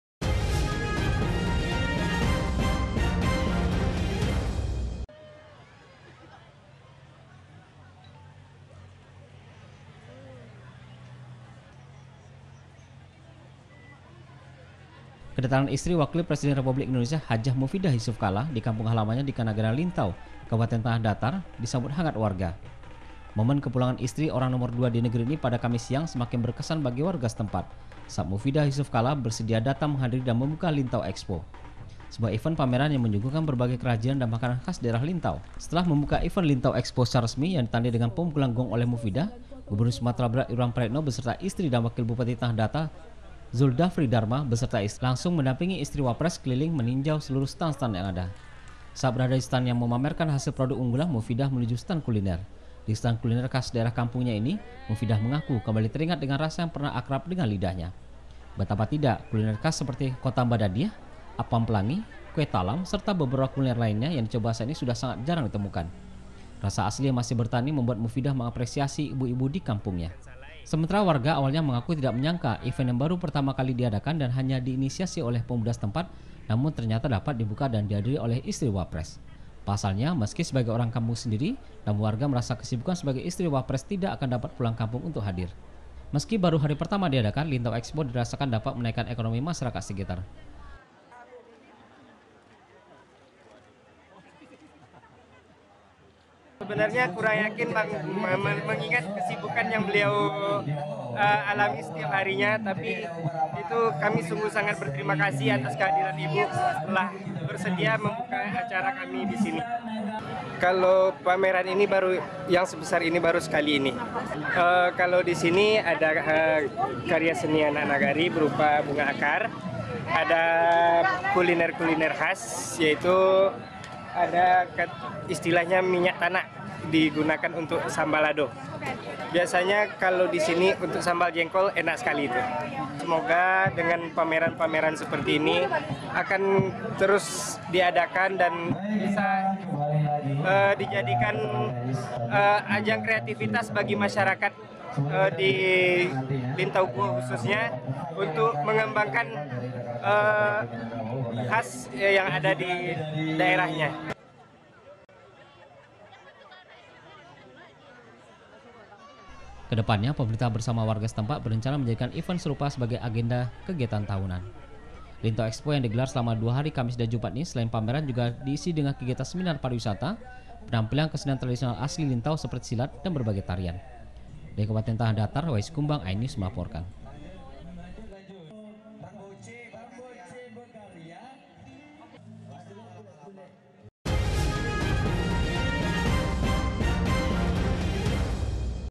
Mufidah Kalla Buka Lintau Expo
Mufidah Yusuf Kalla pulang kampung, dalam kesempatan itu mufidah membuka Lintau Expo yang prakarsai oleh pemuda setempat.